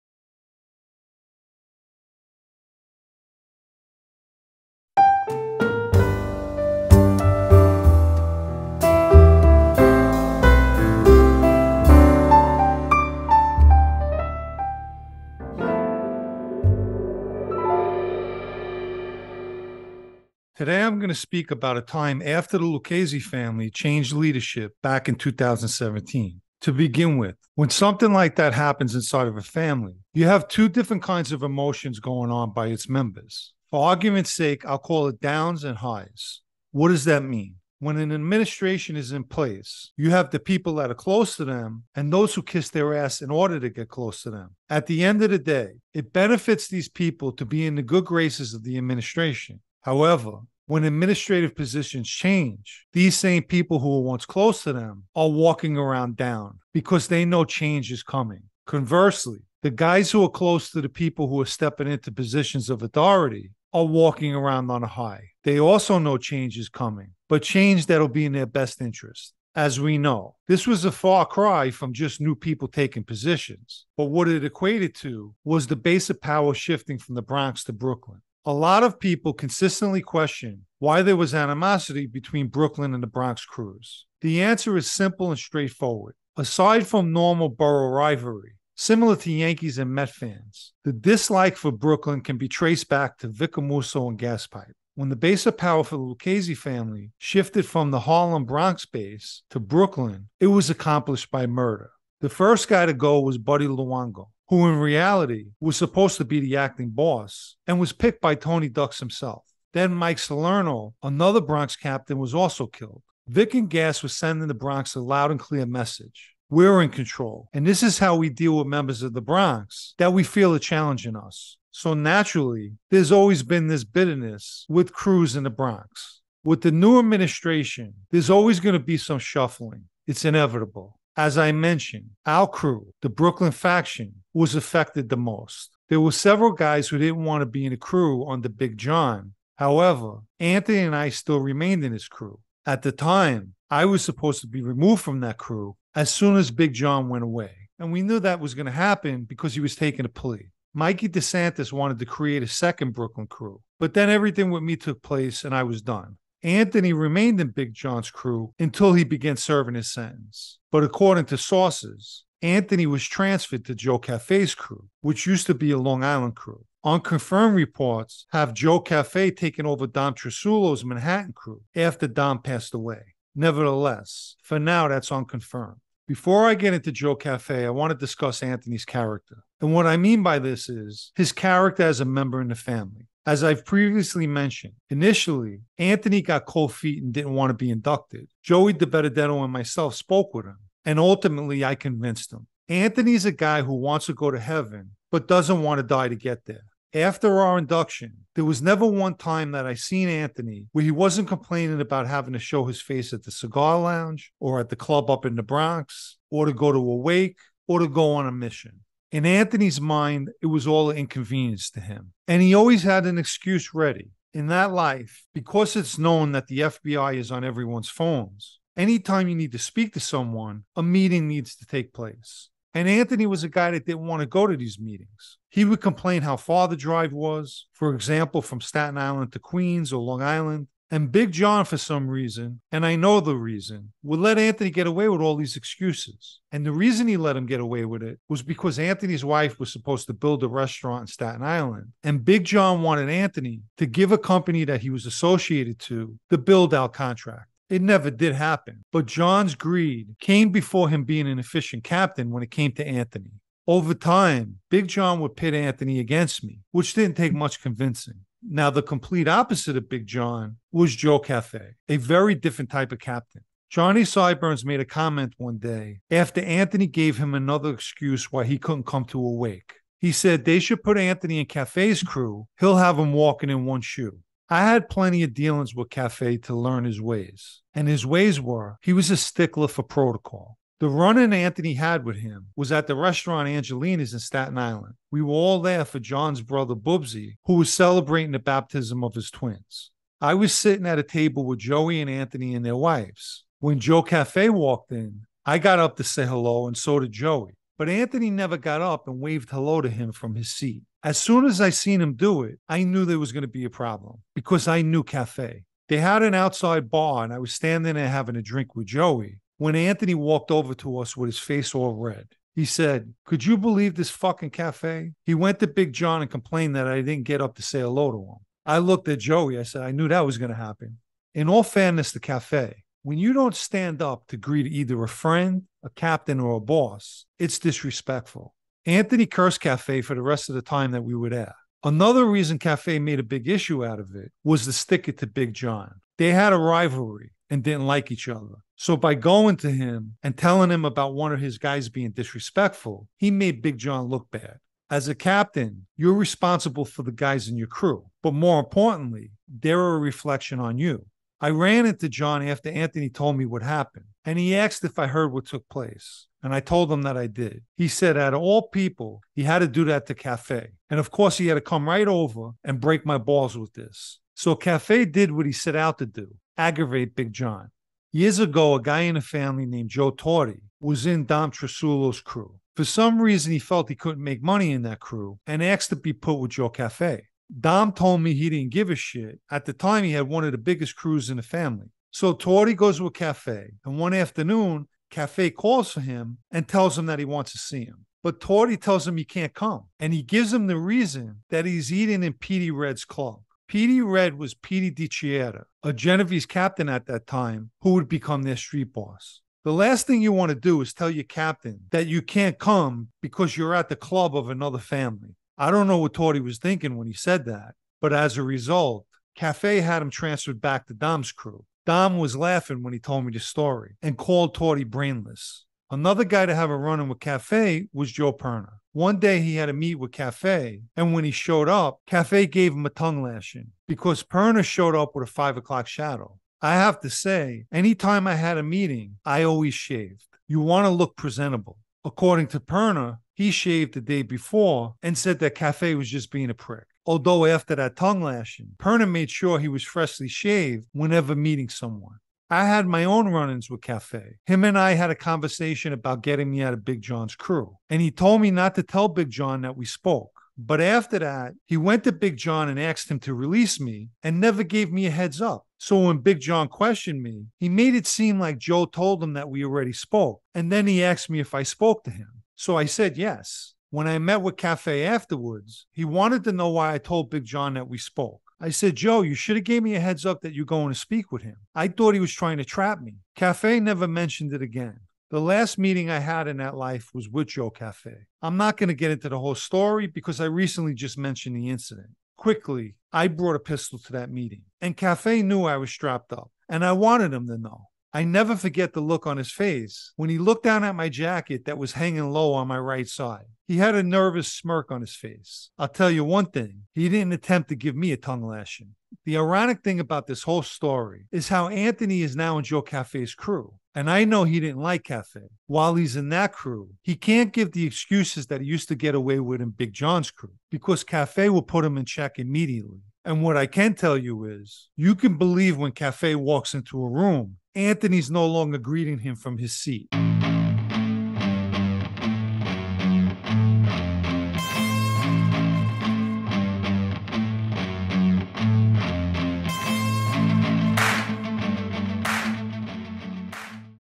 Anthony Guzzo's [ New Crew ]
Speaking about the Lucchese transition in 2017, which caused shuffling within the family. Anthony Guzzo, according to sources, has been transferred to another crew.

If you enjoy reading, take a look at my blog Sitdownnews